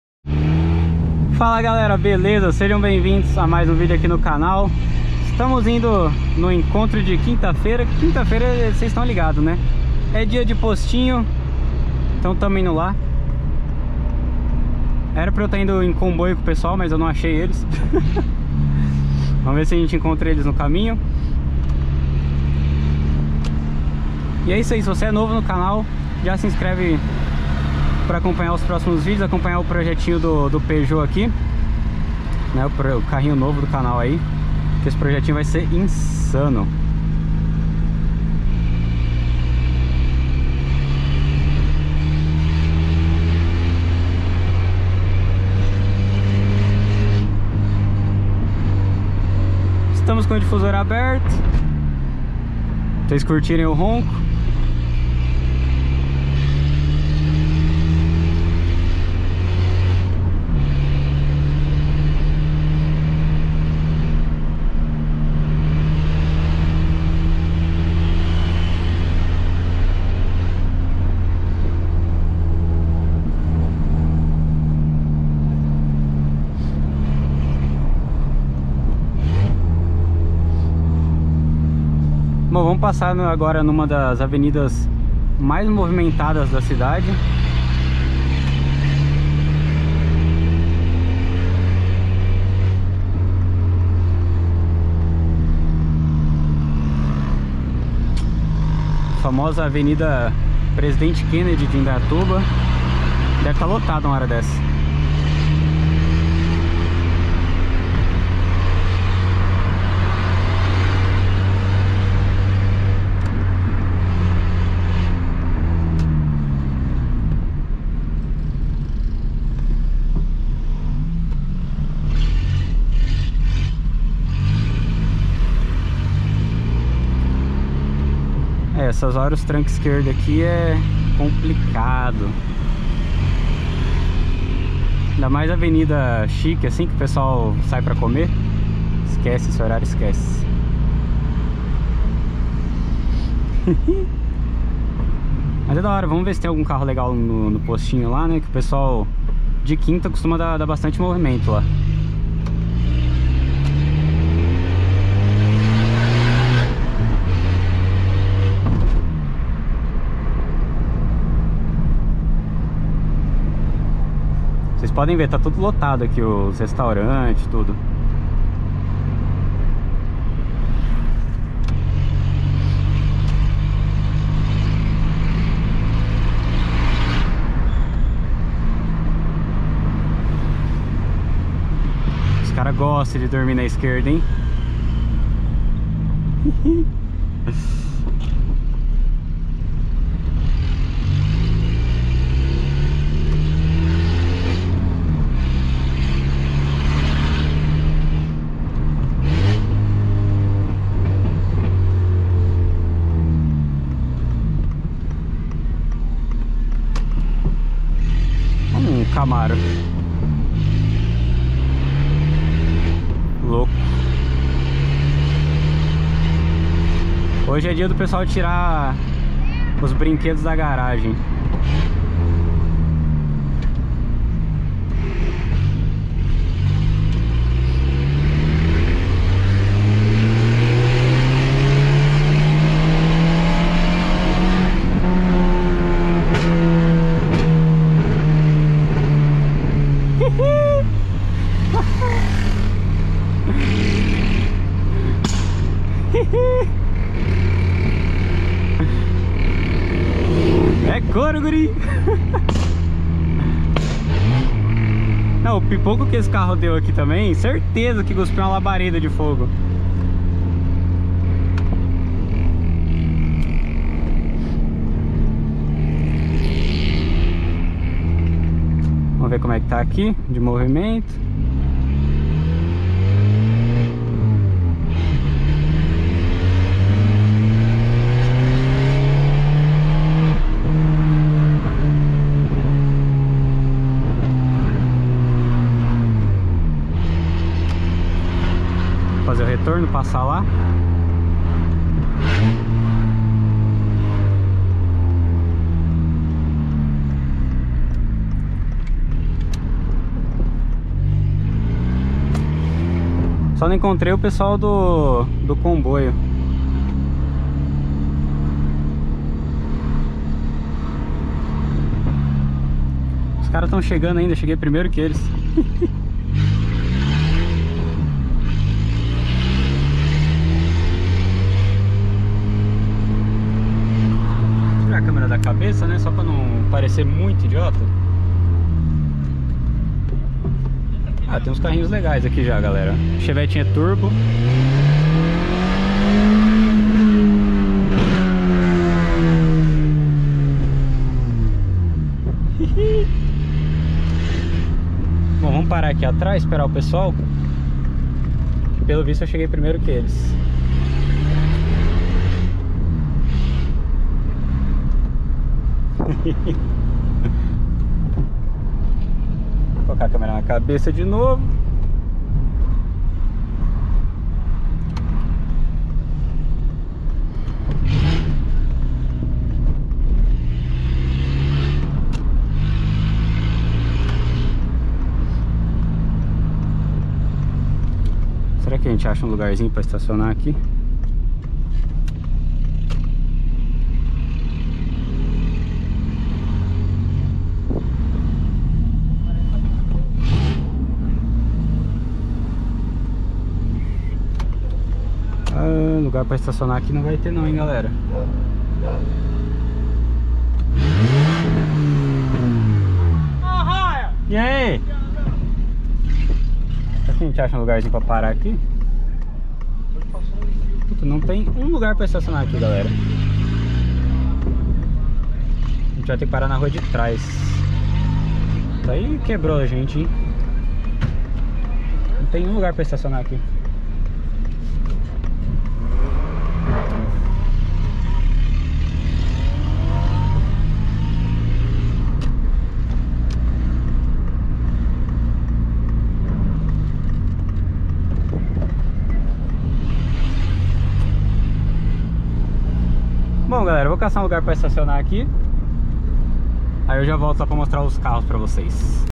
PEUGEOT 206 COM DIFUSOR! PASSEI ACELERANDO NO POSTINHO DE QUINTA 🔥
Fala galera, beleza? hoje é dia de dar um rolê com nosso peugeot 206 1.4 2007, confira!

***SE TORNE MEMBRO DO CANAL E PARTICIPE DO NOSSO GRUPO DO WHATSAPP!***

Itens que utilizamos em nossos carros:

Abafador esportivo JK
Filtro de ar esportivo racechrome

PEUGEOT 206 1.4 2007 ROLÊ NOTURNO 🔥 MONTANA DESAFIOU!